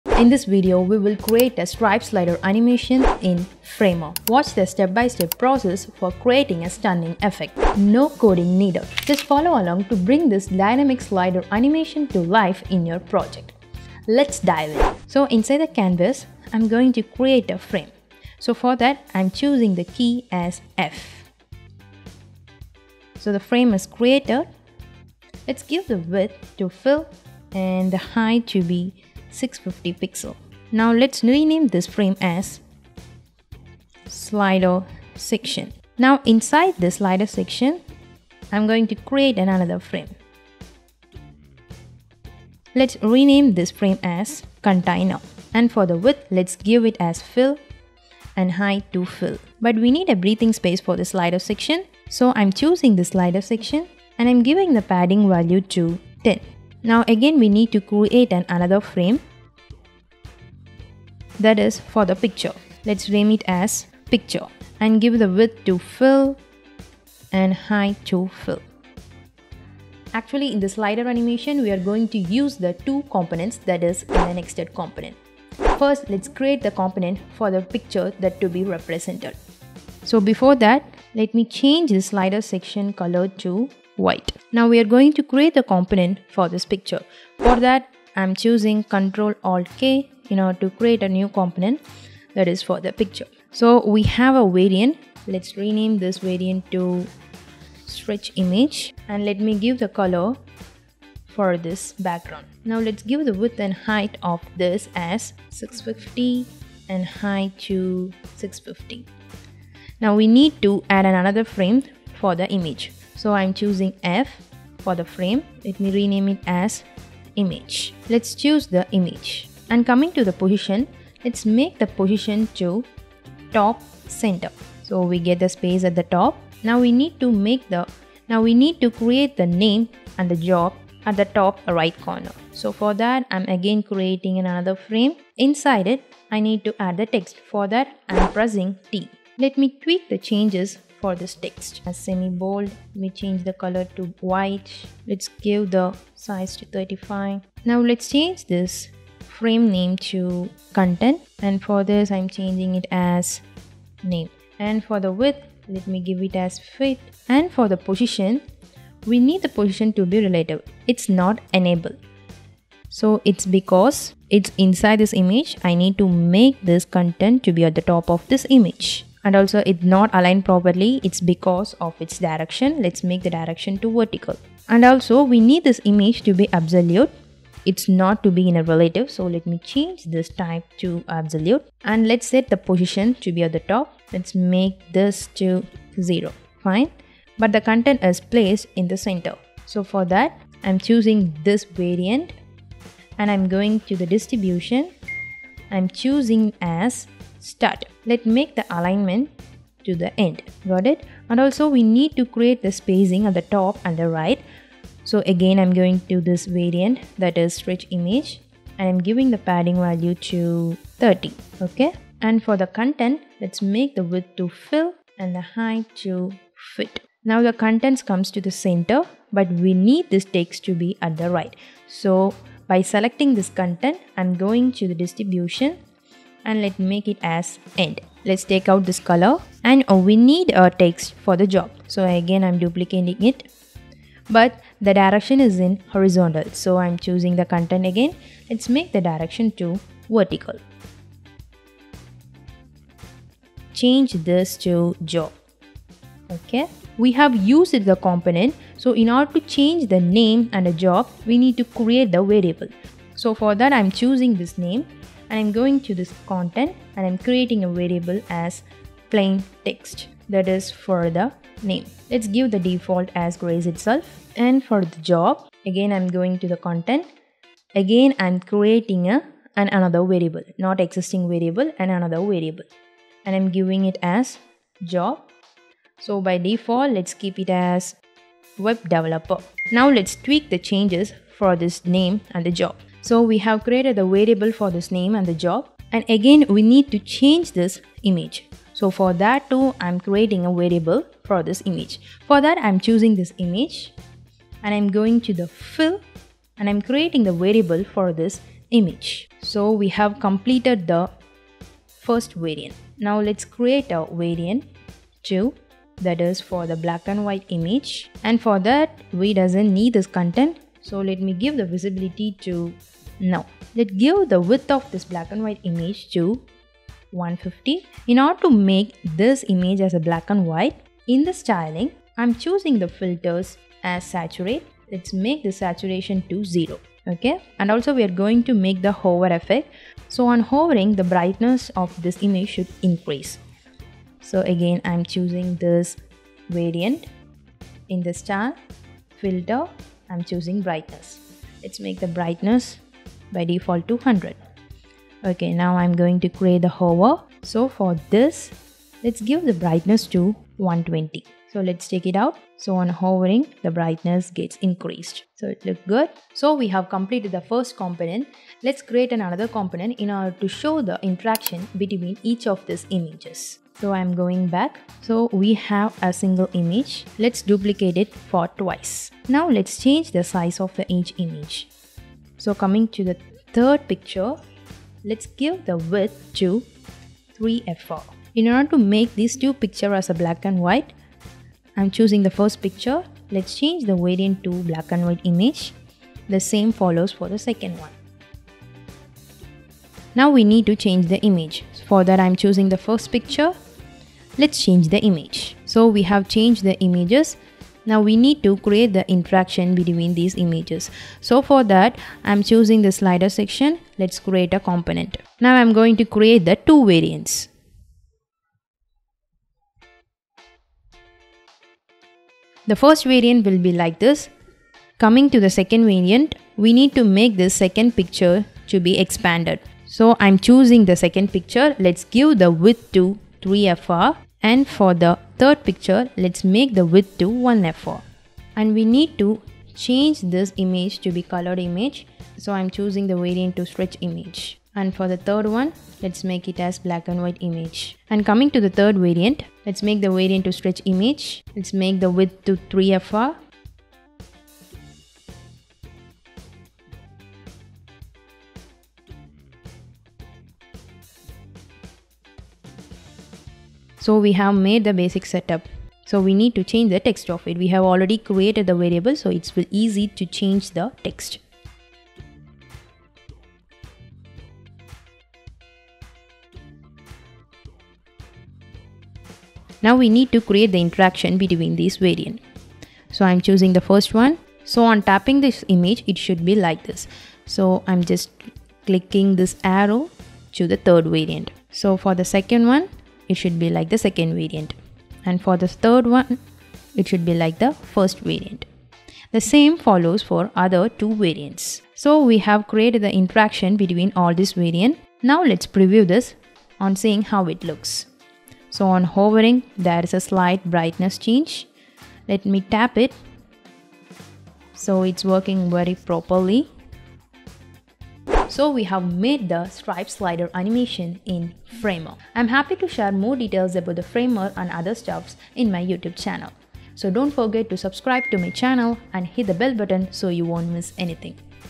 LEVEL UP Your Website with STRIPE SLIDER ANIMATION in Framer
Welcome to our easy-peasy tutorial on making your website look super cool using Framer, a no-code tool that's a total game-changer! In this video i'm going to show you how to add a slick Stripe slider animation to your site without any fancy coding stuff – it's like magic, but way simpler! I discussed about the basic setup, how to create the component and its variant, how to setup the image.

TimeCodes:
00:00 Introduction
00:15 Basic Setup
01:19 First Component
05:03 Variants of First Component
06:03 Setup Of Images
06:43 Second Component
08:16 Preview
08:28 Conclusion

Stay in the loop! Turn on notifications for our latest videos.